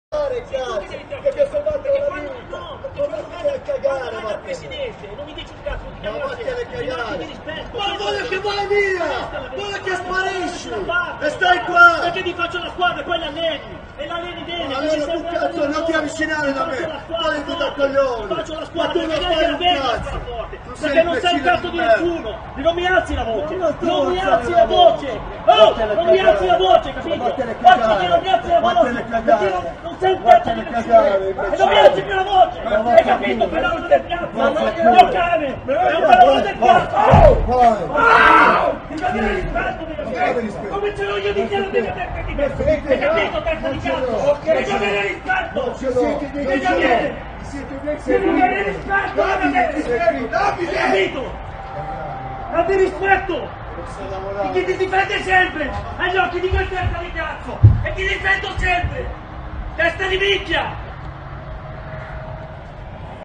Torino, la furiosa lite tra Vagnati e Juric in versione integrale: insulti e spinte, rissa sfiorata
L'incredibile lite tra il direttore dell'area tecnica del Torino, Davide Vagnati, e l'allenatore Ivan Juric, ripresa dal balcone di un albergo. I due si sono affrontati nel parcheggio dell'albergo che ospita la squadra. Insieme a Vagnati e Juric era presente il team manager Marco Pellegri, che li ha divisi a fatica. La clamorosa discussione potrebbe essere stata scatenata dall'allenatore croato, scontento per non aver ancora ricevuto i rinforzi richiesti nel calciomercato.